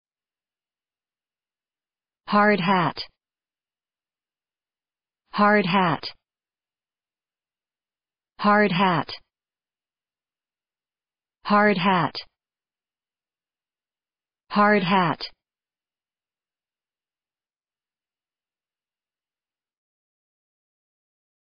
How to pronounce hard hat
hard hat American English pronunciation. How to pronounce hard hat correctly. How to say hard hat in proper American English.